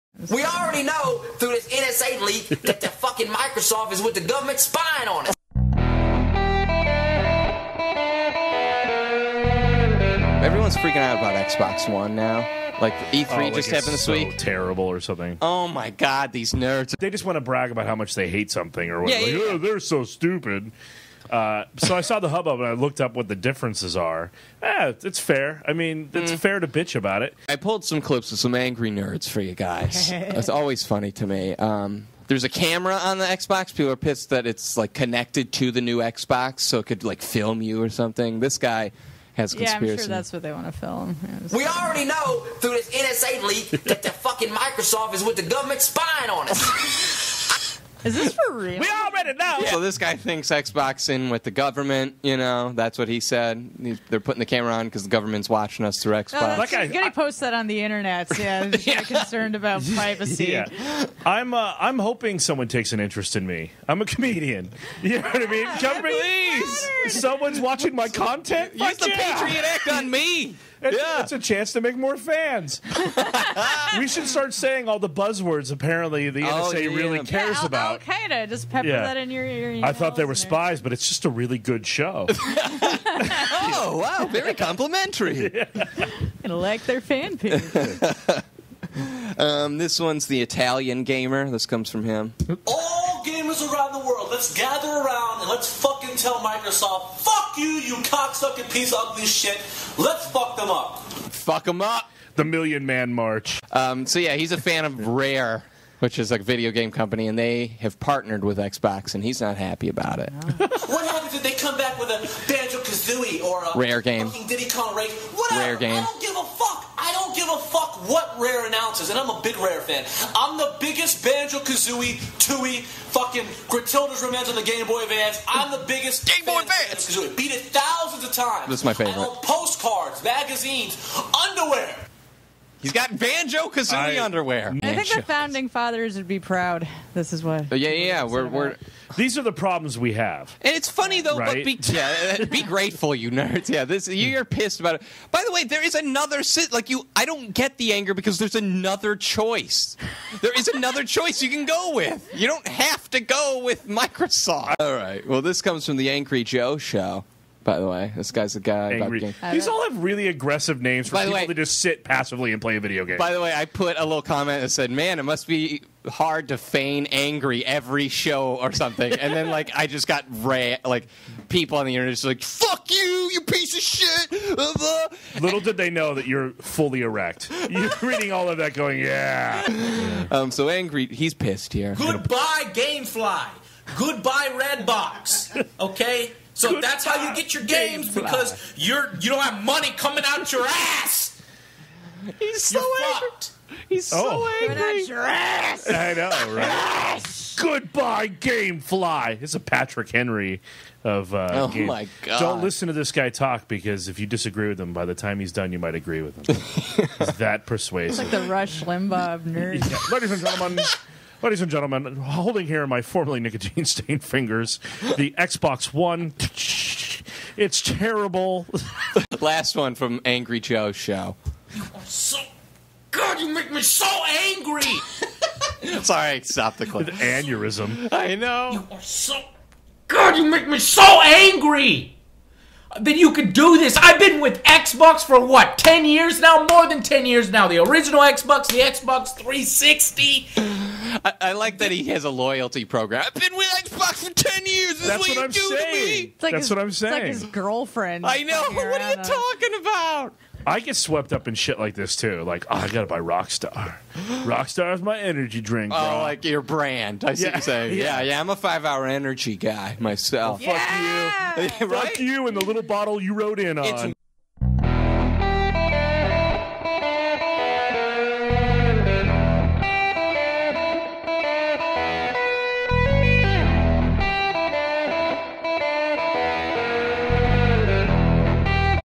Angry Gamers - Self Indulgent Podcast Ep 158
Gamers are irate over the Xbox One and we're going to laugh at how mad they get.